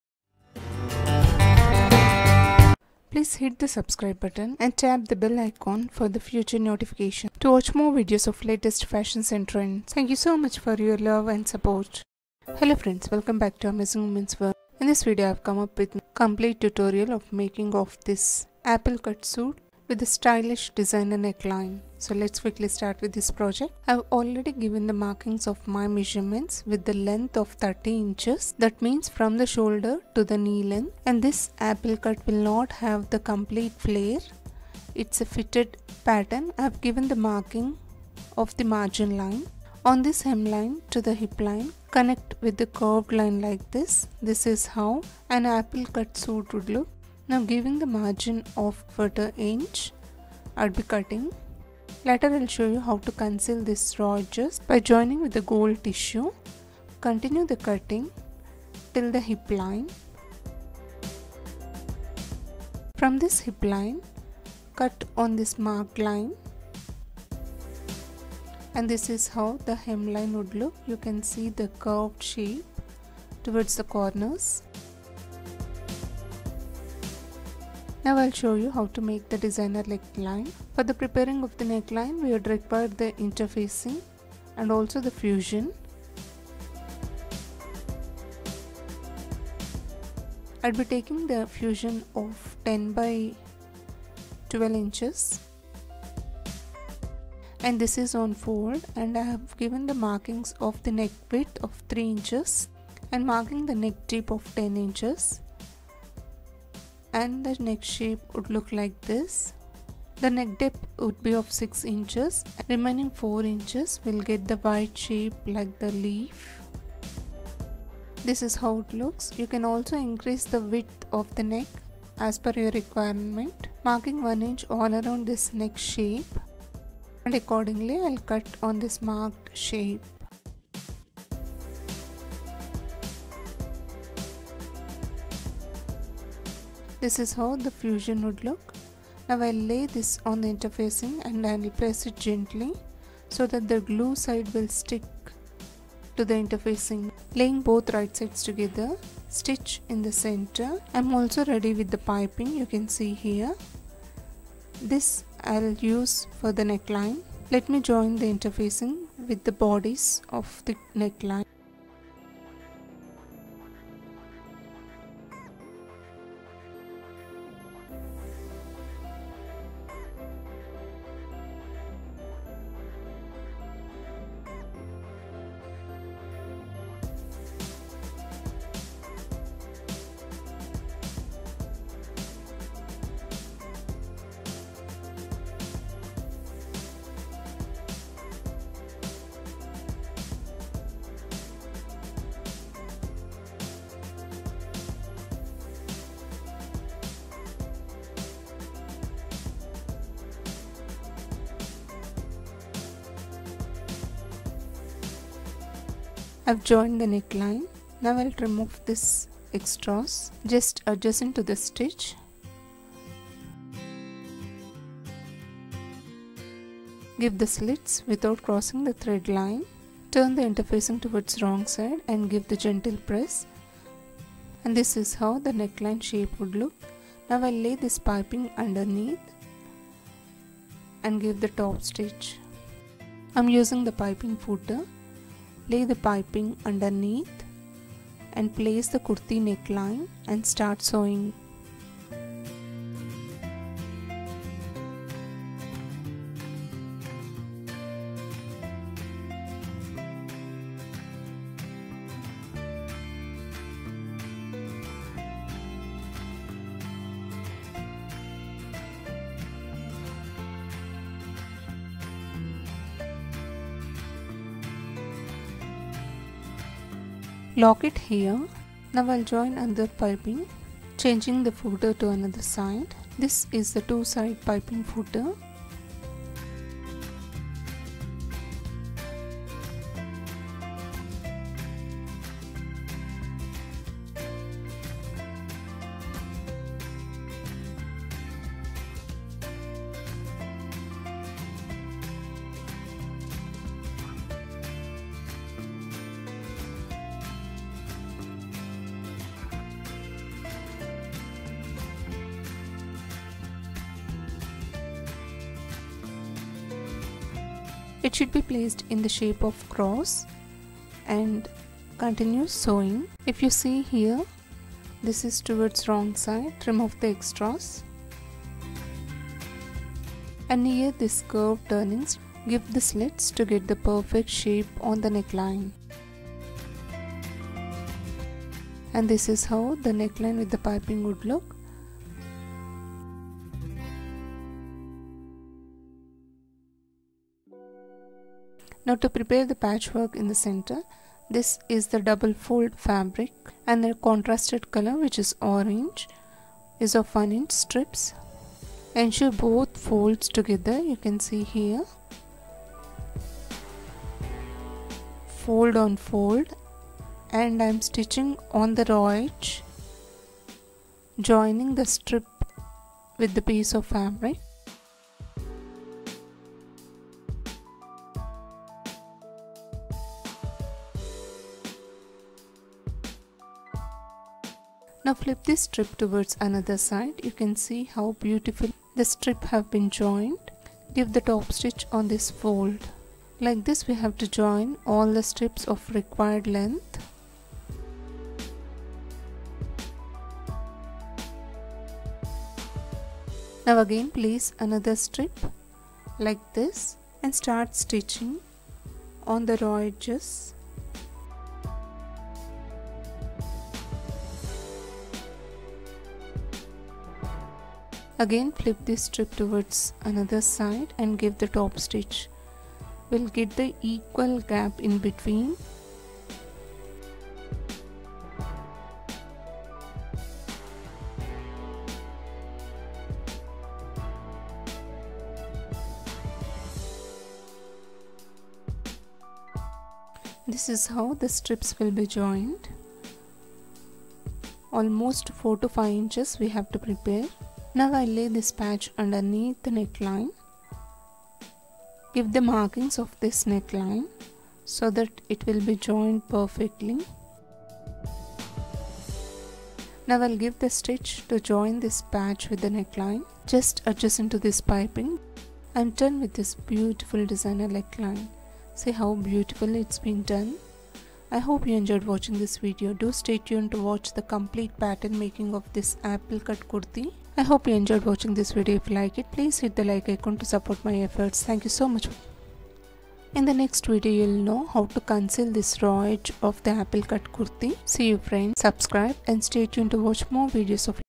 Designer Apple Cut Kurti with Stylish Neck Design Cutting & Stitching-Very Simple & Easy making-DIY
Zerodha Designer Apple Cut Kurti with Stylish Neck Design Cutting and Stitching-Very Simple & Easy making at home- DIY